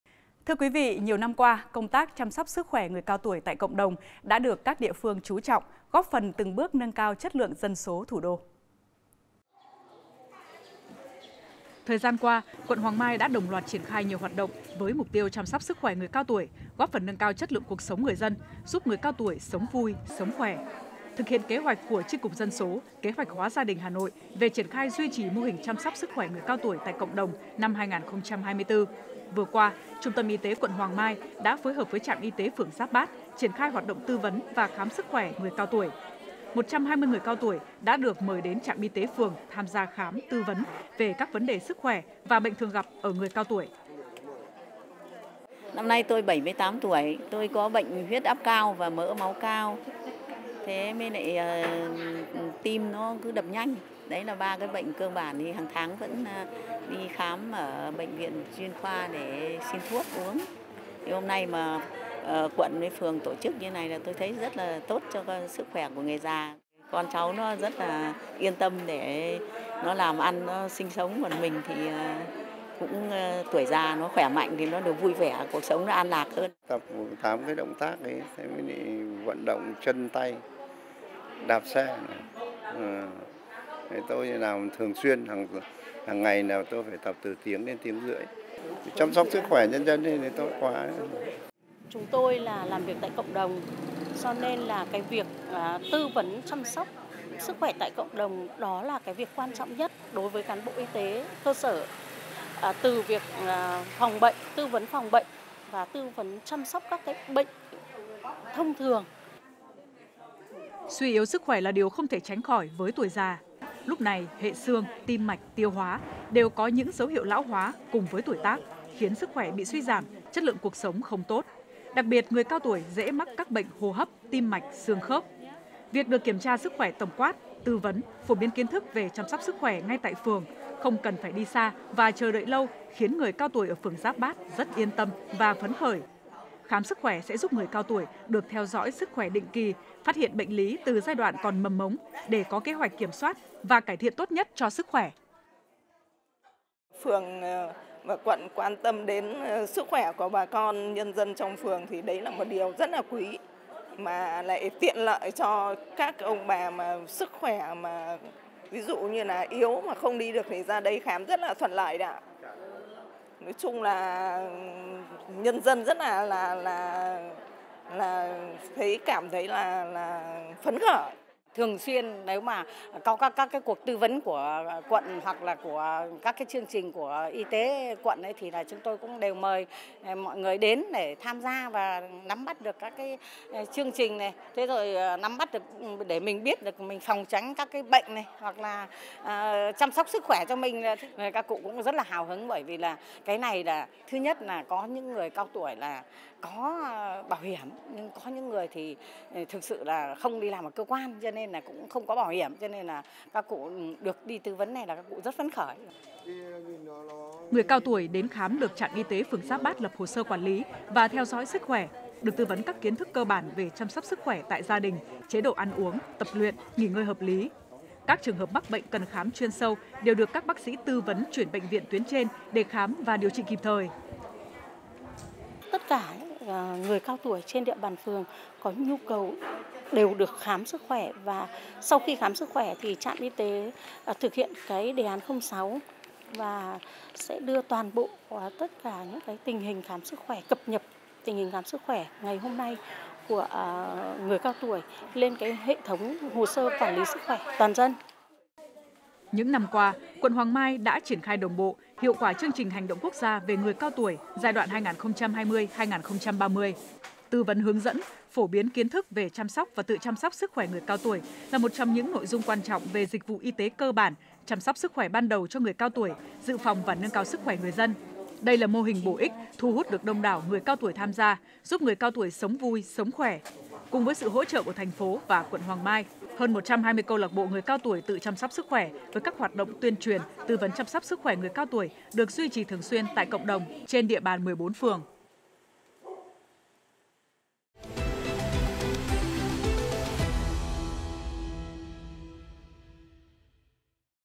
Chăm sóc sức khoẻ người cao tuổi tại cộng đồng | Vì chất lượng dân số Thủ đô
Mô hình chăm sóc sức khoẻ người cao tuổi tại cộng đồng ngày càng nhân rộng và phát huy hiệu quả, góp phần nâng cao chất lượng dân số Thủ đô.